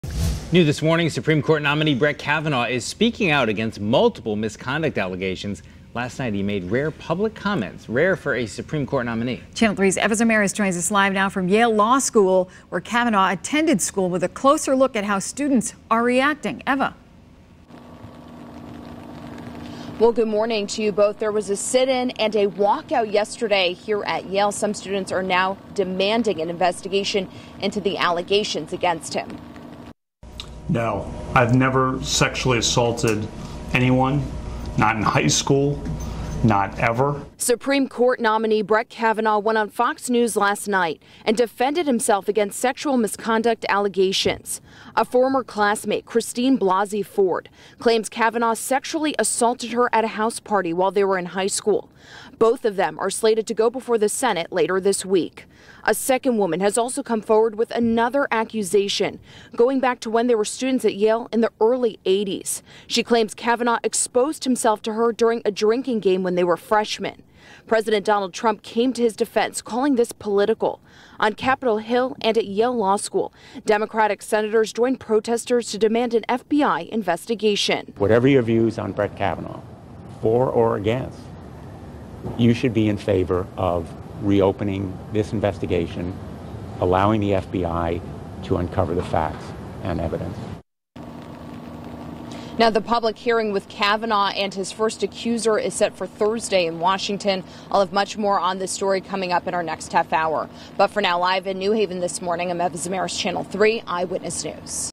Kavanaugh says he's never sexually assaulted anyone
Brett Kavanaugh is speaking out now that two people are accusing him of sexual assault.